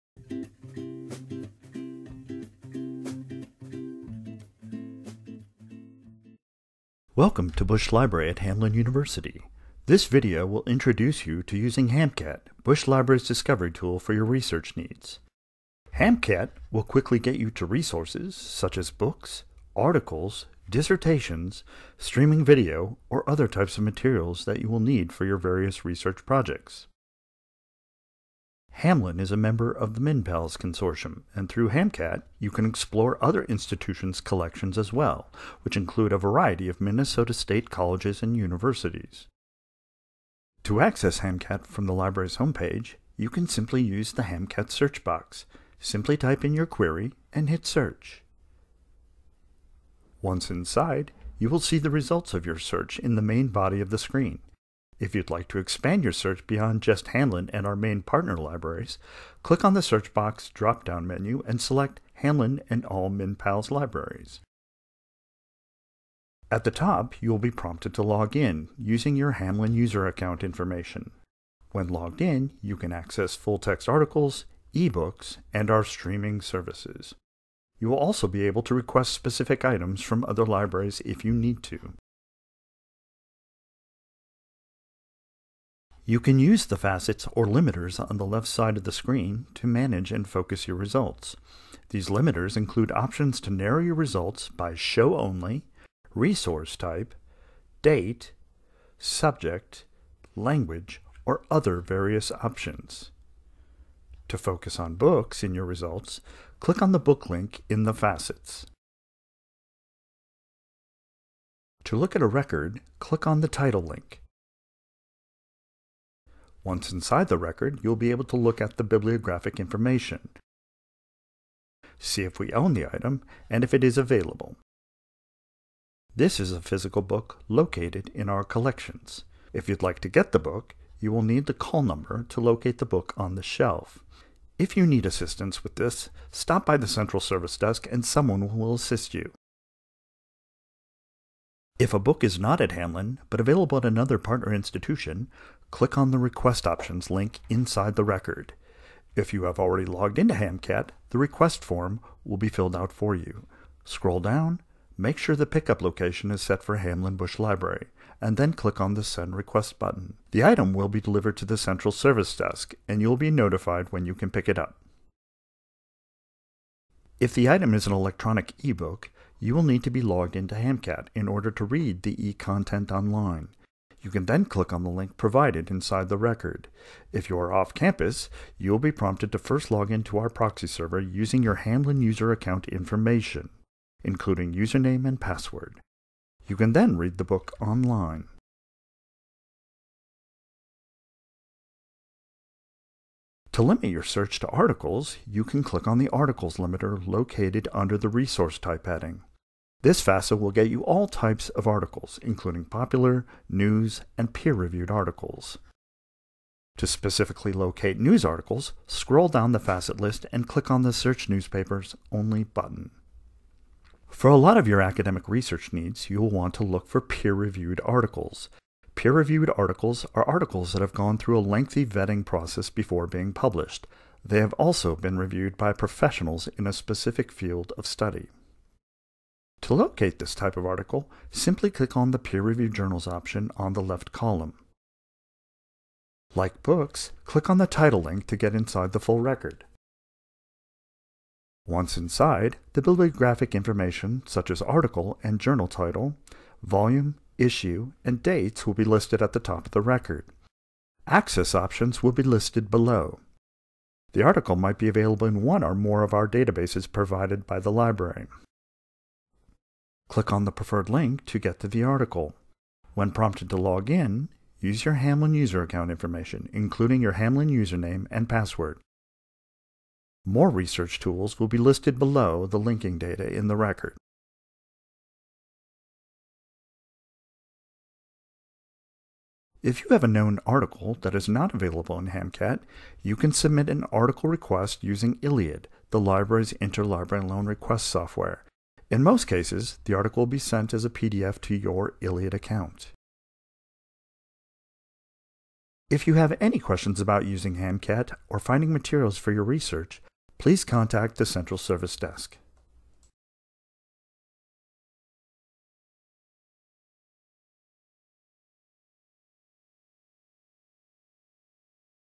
Introduction to HamCAT
A introductory video to Bush Library's main discovery tool, HamCAT. Use HamCAT to discover materials in the library, including books, articles, videos and other materials.